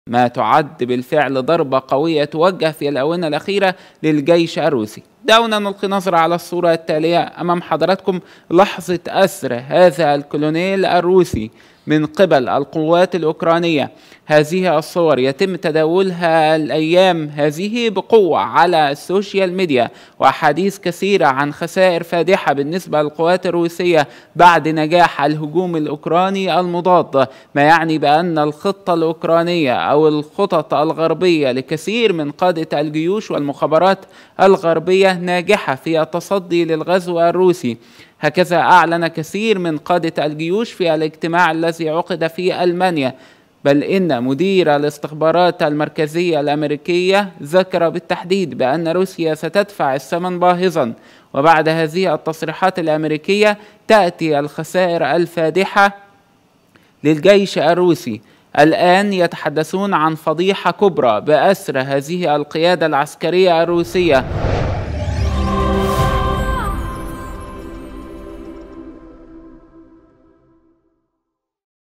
شاهد.. لحظة أسـر قيادة عسكرية روسية كبيرة في أوكرانيا
Nilesat (النايل سات): 11603 أفقي